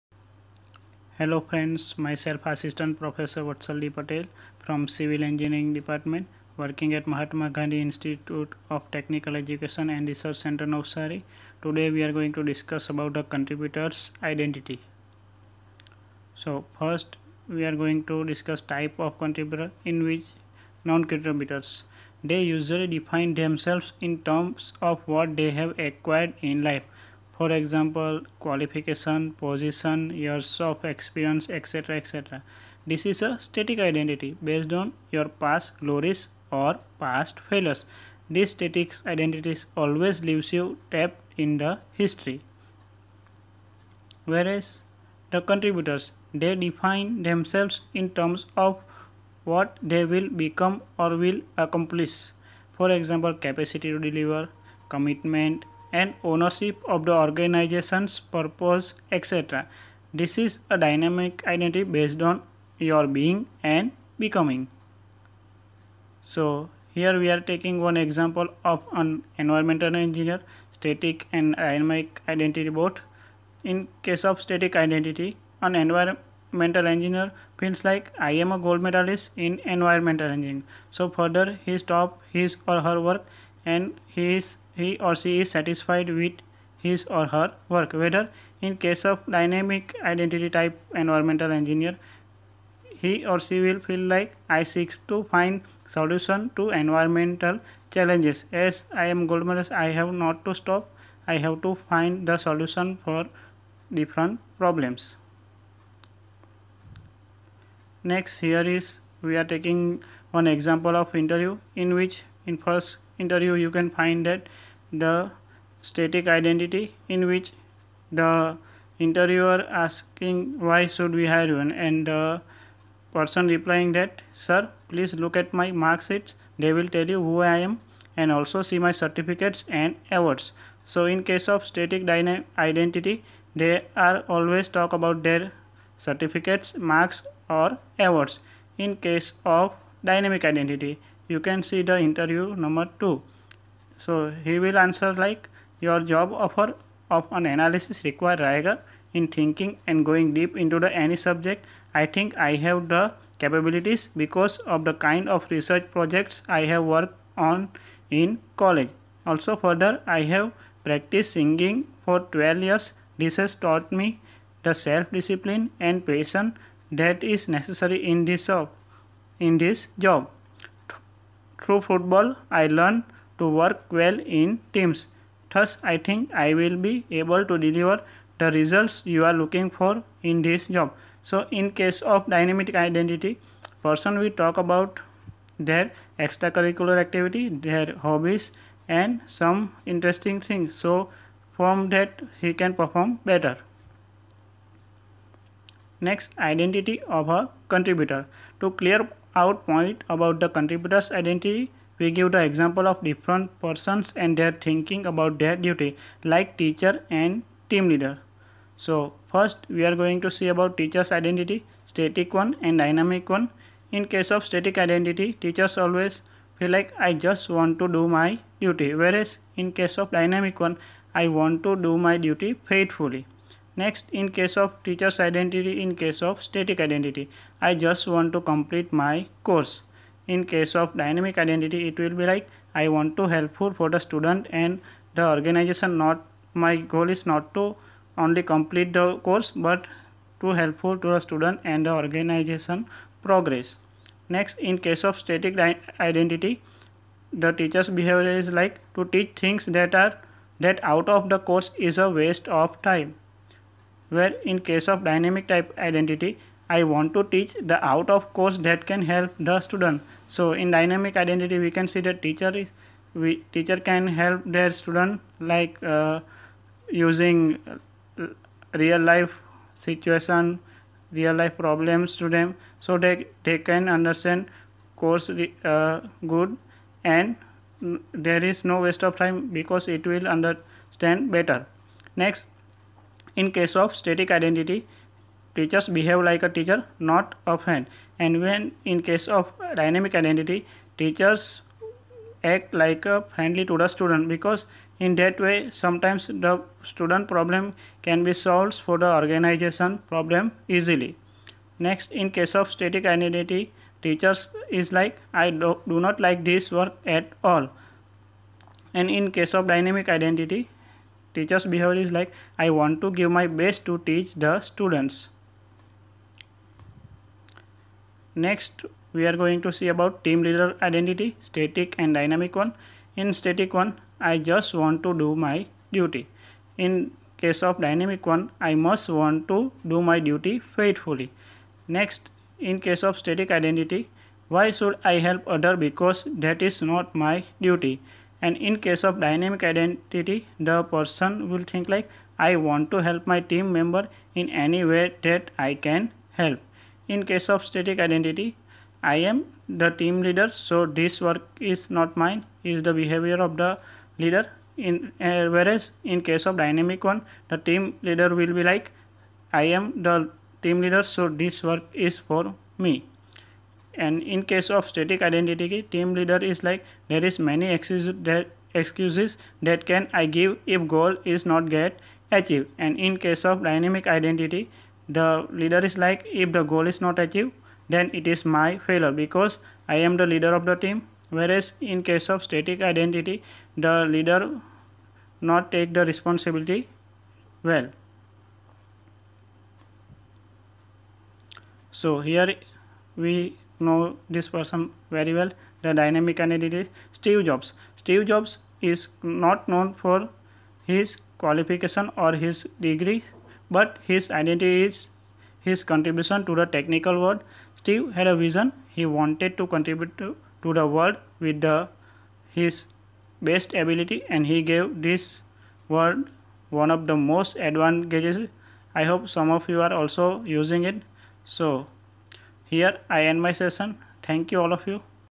THE CONTRIBUTOR'S IDENTITY ~ GTU : Contributor Personality Development (2990001) @MGITER, NAVSARI
Student develops his/ her own answer to the question “who am I?” The student becomes aware of the fact that Non-contributors usually define themselves in terms of what they have acquired in life (e.g. qualifications, position, years of experience, etc.) while Contributors define themselves in terms of what they will become or accomplish (e.g. capacity to deliver, commitment and ownership of the organization's purpose, etc.)